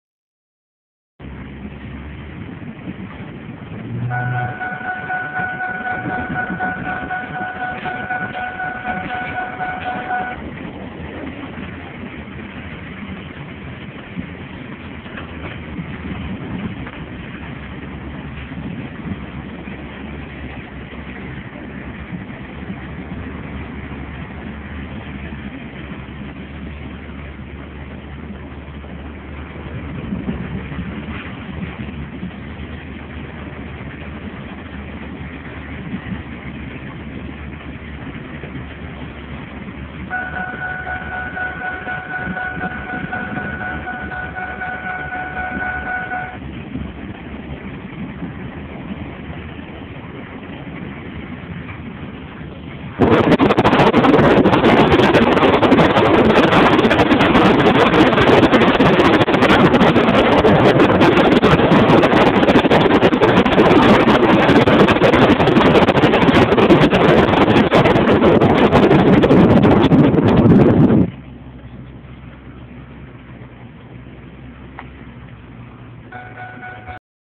Drift Trikes - First Ride - Chris & Joe
VID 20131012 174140 - Chris & Joe - First ride on "Drift Trikes" down Snake Road.  Sorry for the poor audio quality.